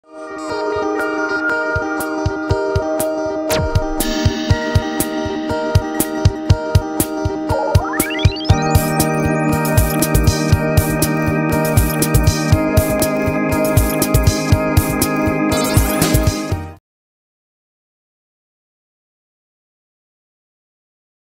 birch tree intro music and words
A little something i did in After Effects using a 3-d plain, and a camera.